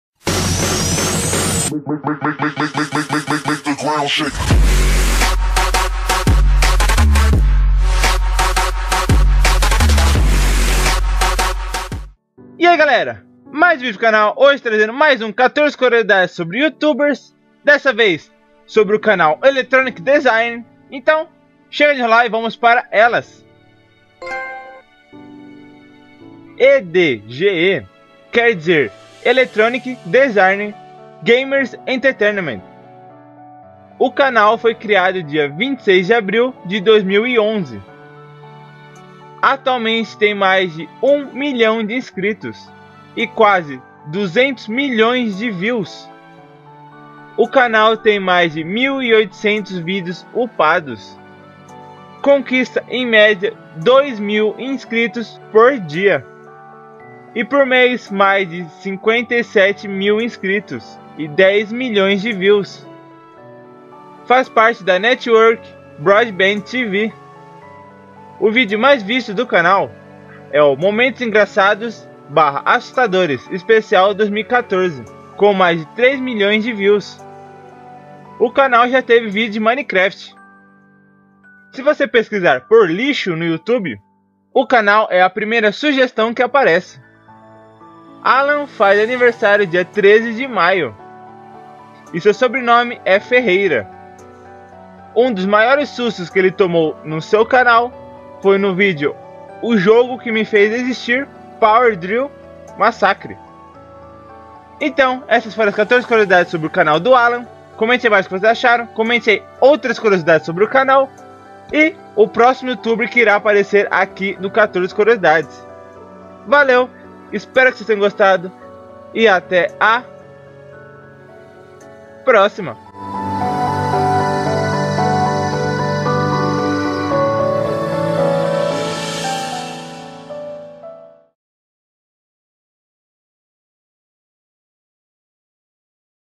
14 Curiosidades sobre YouTubers: ElectronicDesireGE
Hoje trazendo mais 14 curiosidades sobre o canal do Alan Ferreira ou Alanzoka, ElectronicDesireGE